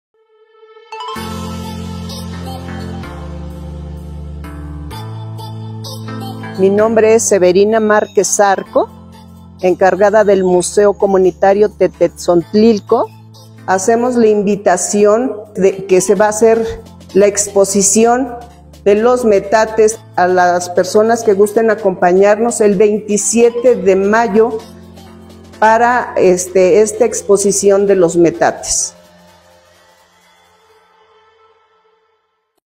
No te pierdas el Primer Festival del Metate en Tizayuca
HidalgoTieneAlgo En Tizayuca el Primer Festival de Cocina Tradicional y uso del Metate “Metlatl”.
Agenda próximo sábado 27 de Mayo a partir de las 10:00 de la mañana en la comunidad de Olmos.